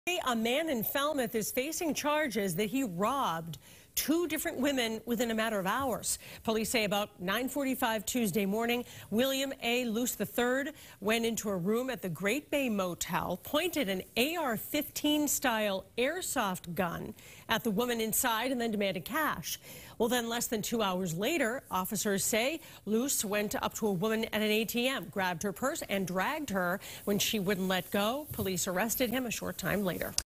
East Falmouth Man Charged With Robbing Women At ATM, Motel
William Luz, 51, of East Falmouth is facing several charges including robbery and assault and battery with a dangerous weapon.